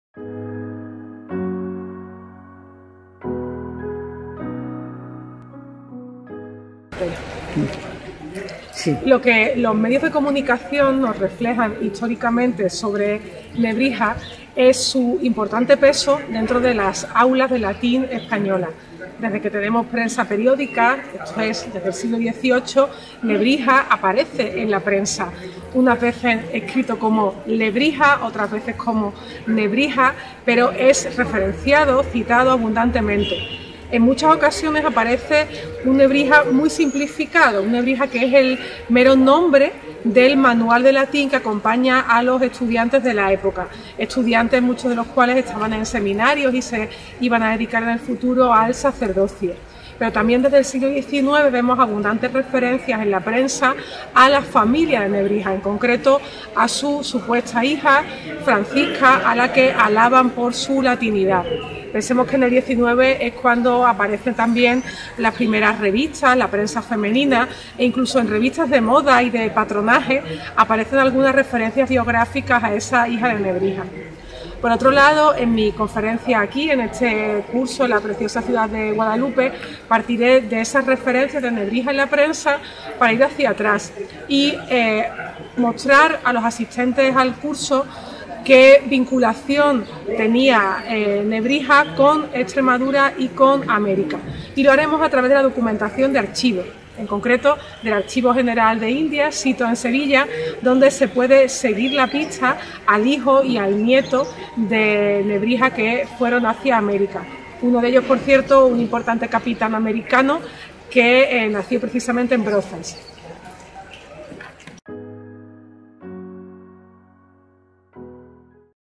Entrevista a Lola Pons Rodríguez
¿Qué reflejan los medios de comunicación sobre Nebrija?

ELIO ANTONIO DE NEBRIJA: DE EXTREMADURA A AMÉRICA
Guadalupe (Cáceres). Del 27 al 29 de julio de 2022